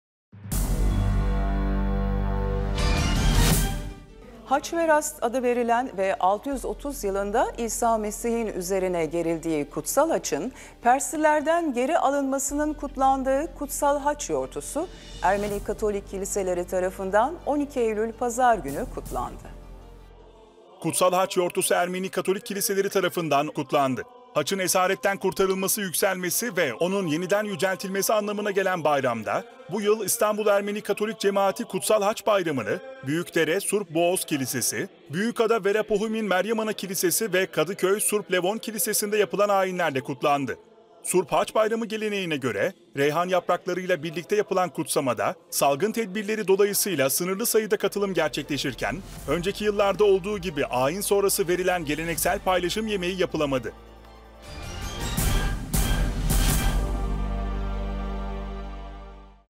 Ermeni Katolik Kiliselerinde Kutsal Haç Bayramı | 17.09.2021
“Haçverast” adı verilen ve 630 yılında İsa Mesih’in üzerine gerildiği “Kutsal Haç”ın Perslilerden geri alınmasının kutlandığı Surp (Kutsal) Haç Yortusu, Ermeni Katolik kiliseleri tarafından da 12 Eylül Pazar günü kutlandı.

Haç’ın esaretten kurtarılması yükselmesi ve onun yeniden yüceltilmesi anlamına gelen bayramda; bu yıl, İstanbul Ermeni Katolik Cemaati Kutsal Haç Bayramı’nı, Büyükdere Surp Boğos Kilisesi, Büyükada Verapohumin Surp Asdvadzazin Kilisesi ve Kadıköy Surp Levon Kilisesi’nde yapılan ayinlerle çoşku ile kutladı.

Surp Haç Bayramı geleneğine göre reyhan yaprakları ile birlikte yapılan kutsamada, salgın tedbirleri dolayısıyla sınırlı sayıda katılım gerçekleşirken önceki yıllarda olduğu gibi ayin sonrası verilen geleneksel paylaşım yemeği yapılamadı.